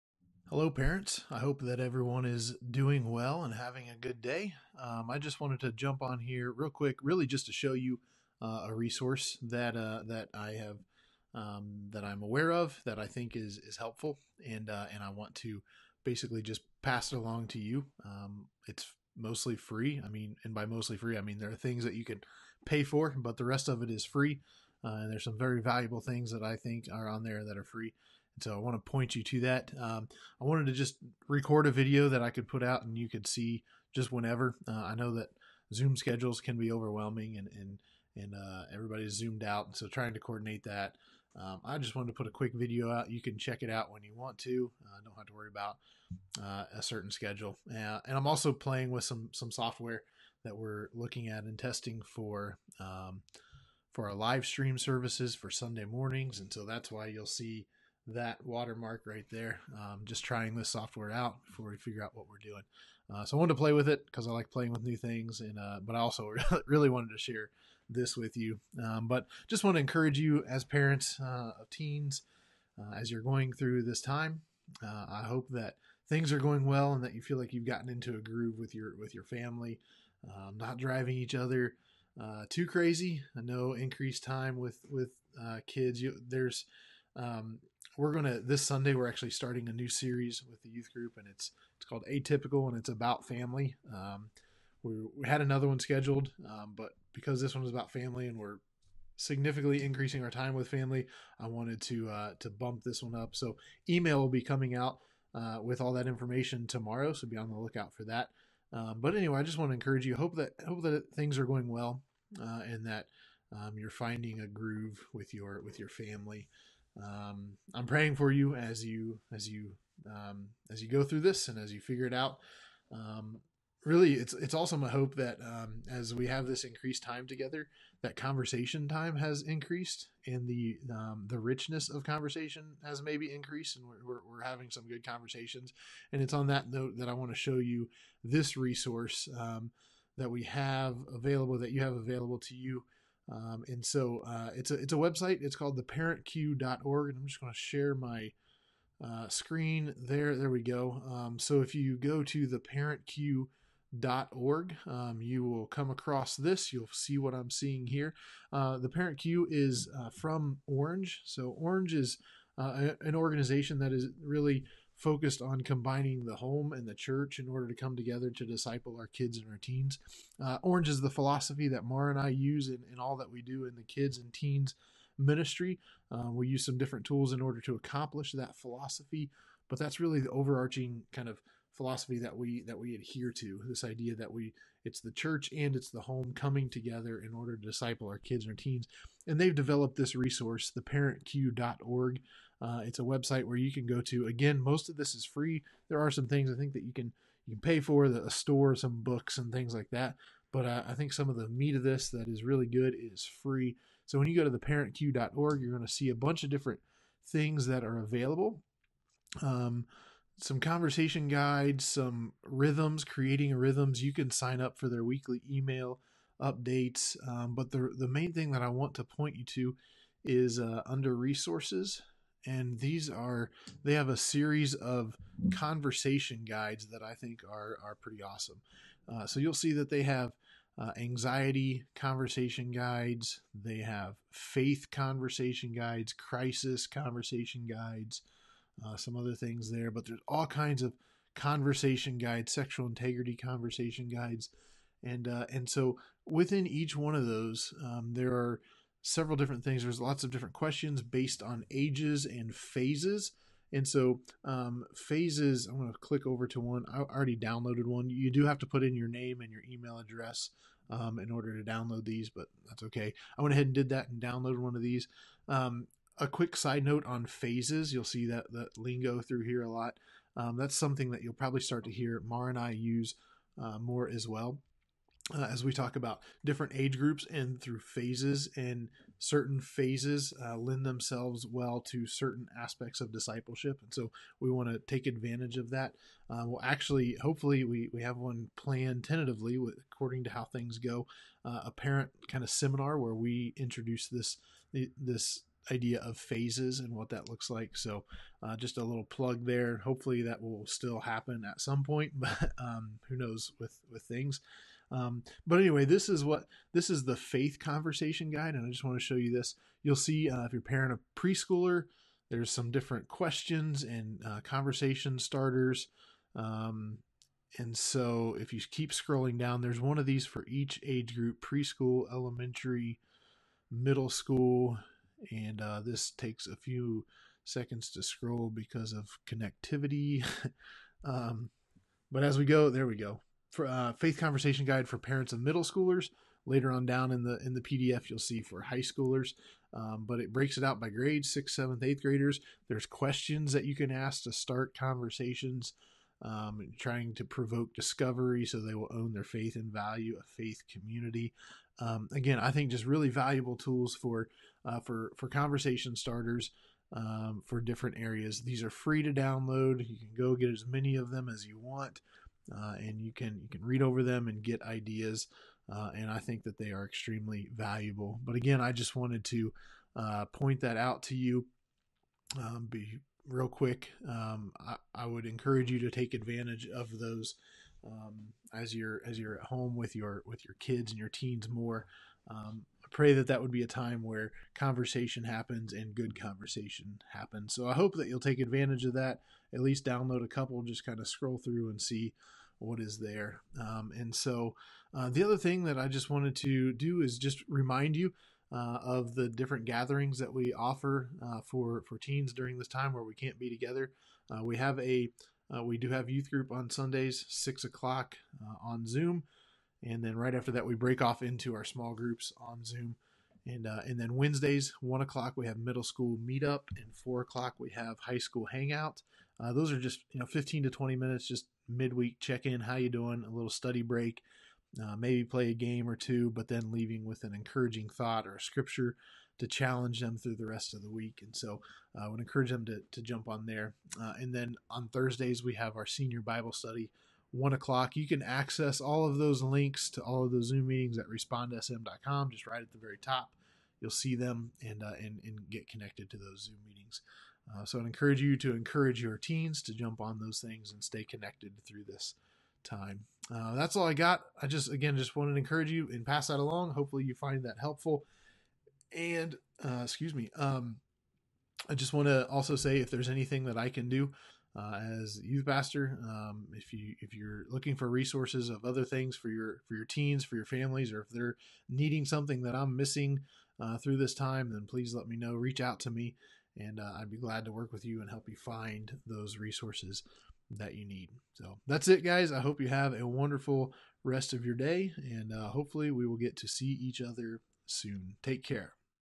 Parent Cue
Pastor Josh talks about a valuable resource for parents.